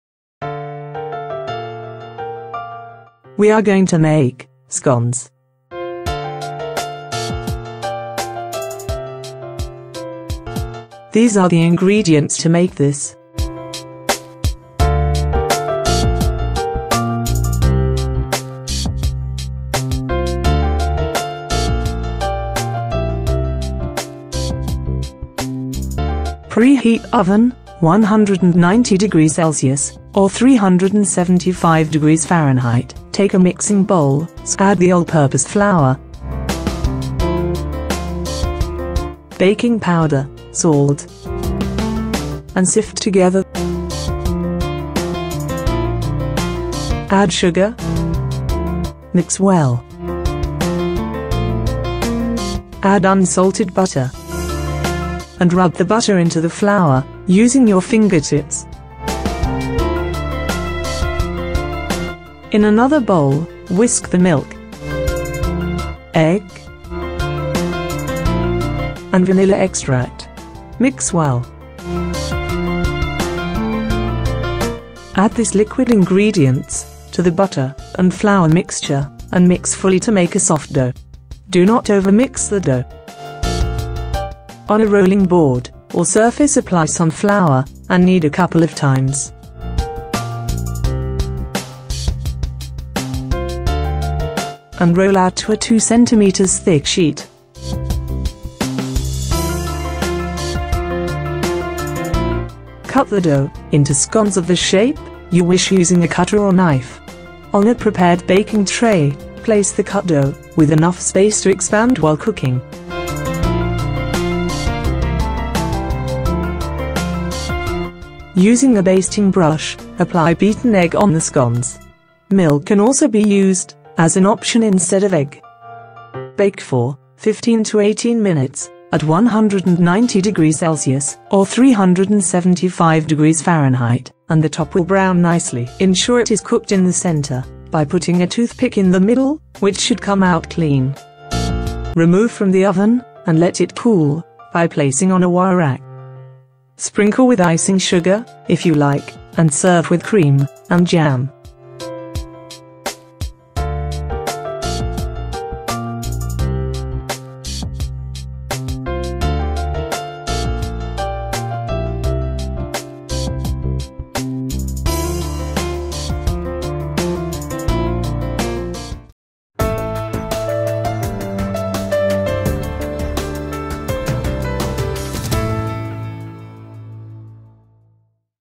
Scones Recipe - Look and Cook step by step recipe | How to make Scones Recipe
SCONES (LC14083): Scones is a type of quick bread and the easiest tea time snack. Scones are made of wheat with baking powder and baked on lined baking sheets and are slightly sweet.

Look and Cook offers recipes with step by step pictures and video instructions to make cooking a hassle free and fun experience.